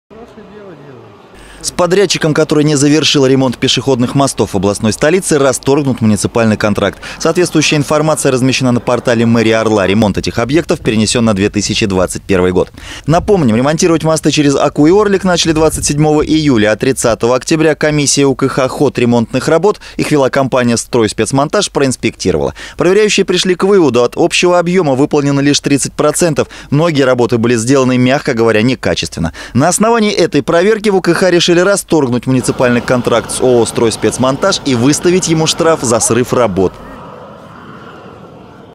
Ремонт пешеходных мостов в Орле отложен на 2021-й
С подрядчиком, который не завершил ремонт пешеходных мостов в областной столице, расторгнут муниципальный контракт. Соответствующая информация размещена на портале мэрии Орла. Ремонт этих объектов перенесен на 2021 год.
Напомним, ремонтировать мосты через Оку и Орлик начали 27 июля, а 30 октября комиссия УКХ ход ремонтных работ - их вела компания «Стройспецмонтаж» - проинспектировала. Проверяющие пришли к выводу: от общего объема выполнено лишь 30%. Многие работы были сделаны мягко говоря некачественно. На основании этой проверки в УКХ решили расторгнуть муниципальный контракт с ООО «Стройспецмонтаж» и выставить ему штраф за срыв работ.